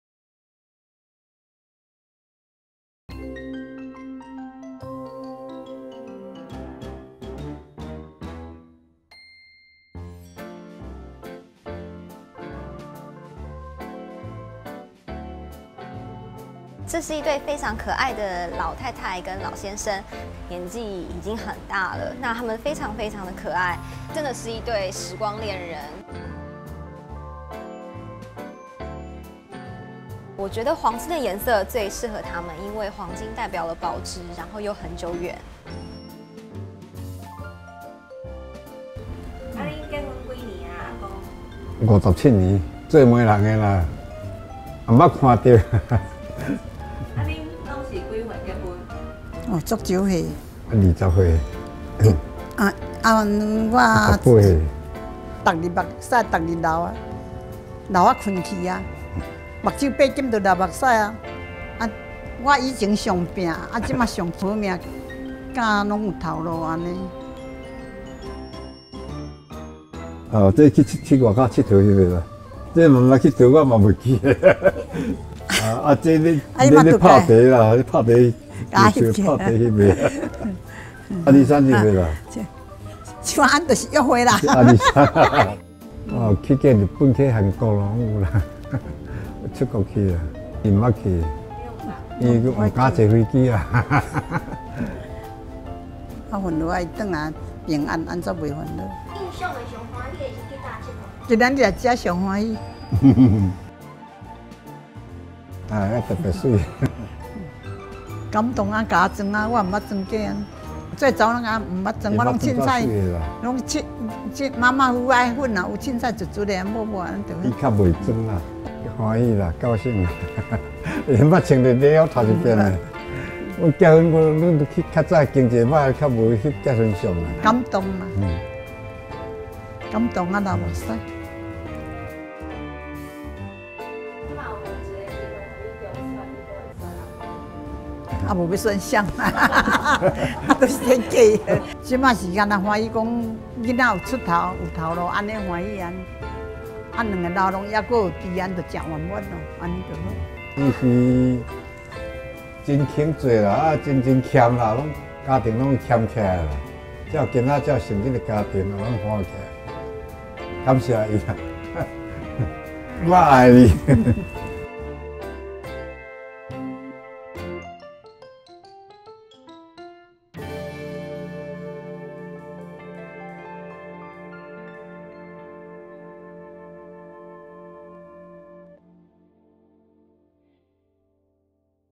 【周大福wedding-幸福的顏色】-暖金色
當愛情經歷長久歲月的淬鍊
會是什麼模樣呢
這對可愛的老公公和老太太
已經結婚57年彼此眼神還是充滿愛意
(偷偷說是爺爺自己報名的喔)
像黃金一樣永遠不變的感情
讓人覺得好溫暖又踏實
快來聽聽他們的感人故事